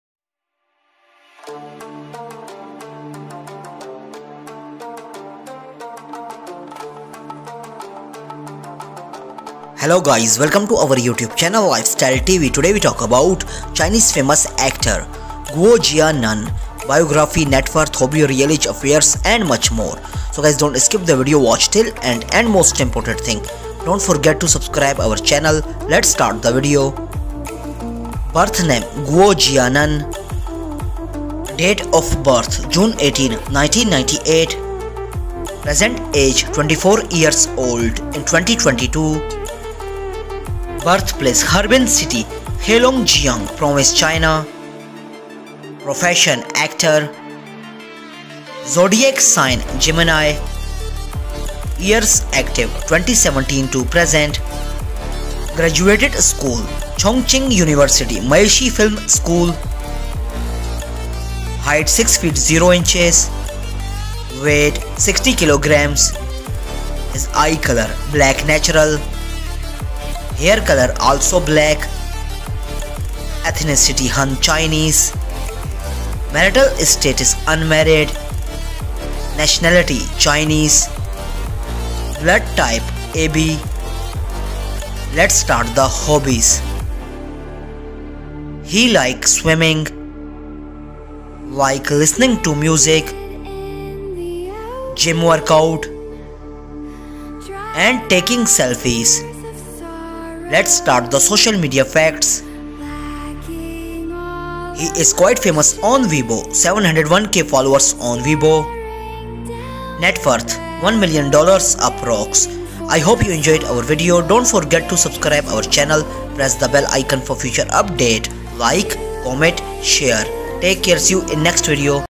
Guo Jia Nan Lifestyle (My Dearest Boss) Biography, Age, Girlfriend, Income, Height, Weight, Facts
Guo Jia Nan Biography (Discovery of Romance) Lifestyle, Hobbies, Age, Girlfriend, Income, Height, Weight, Facts

Guo Jia Nan, born in Heilongjiang, is a Chinese actor who made his acting debut in 2016 with the film "Lei Shen Chuan Qi".

Turkish Translated